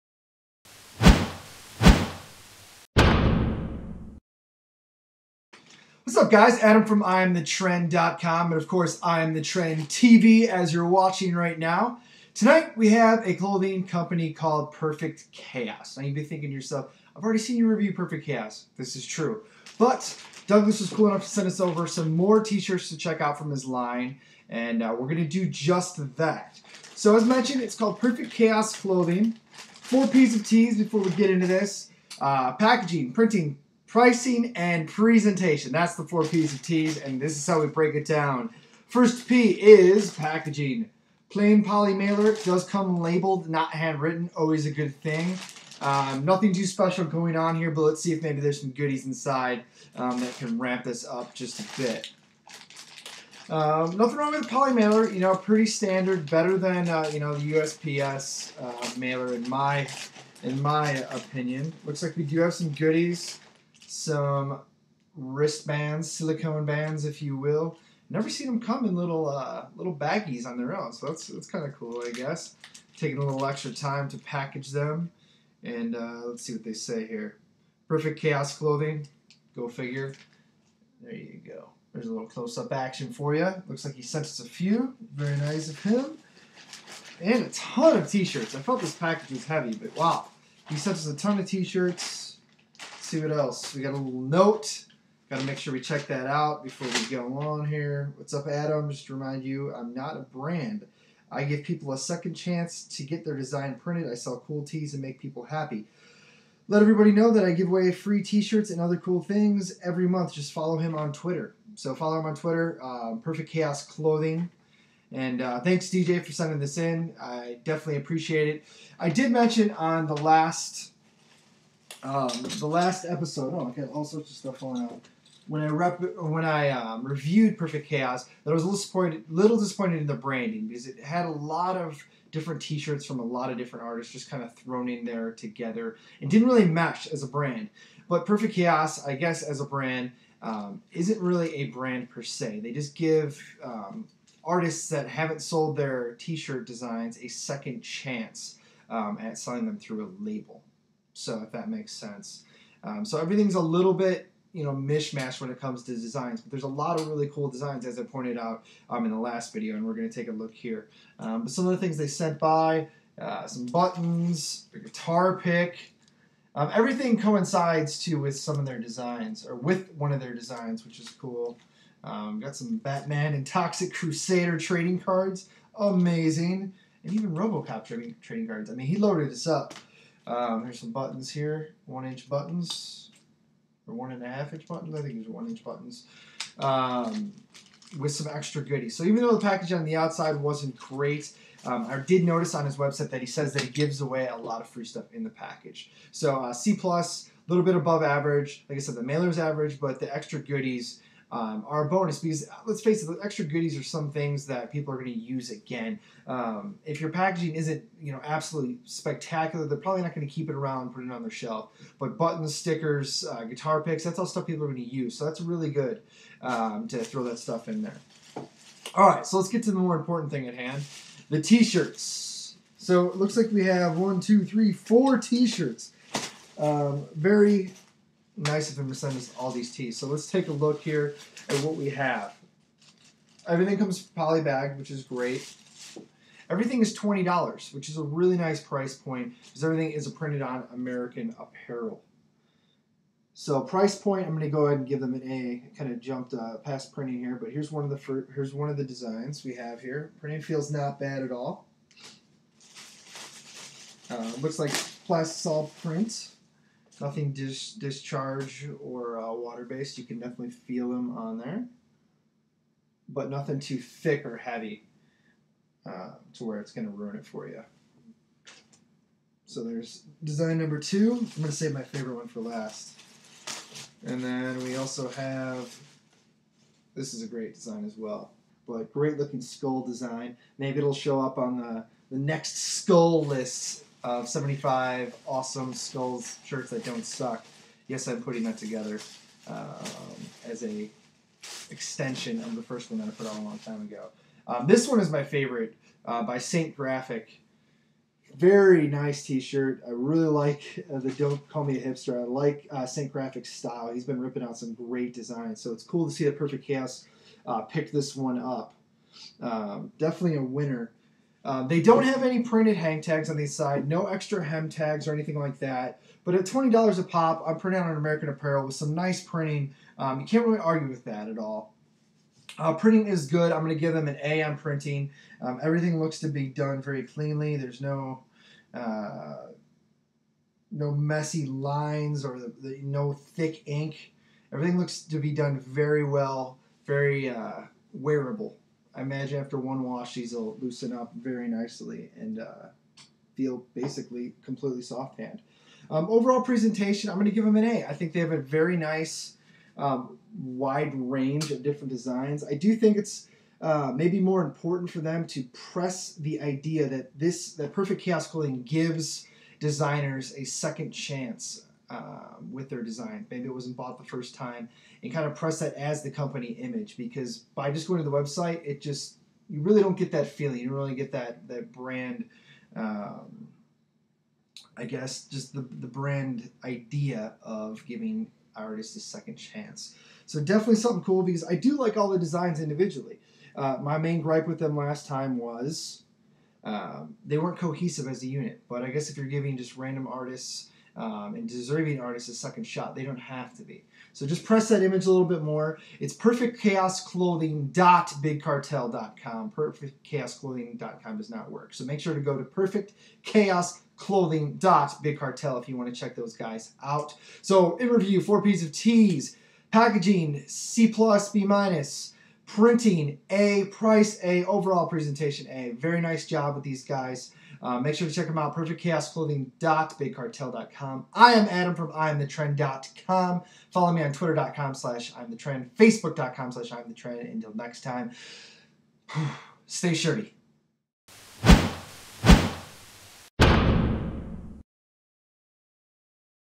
IAMTHTREND TV: Perfect Chaos Clothing Tee Reviews
In this episode of IAMTHETREND TV we give Perfect Chaos Clothing a second look.  We also further examine their concept of giving artists a second chance with their designs by forming a clothing line that only prints artwork that has been passed on before..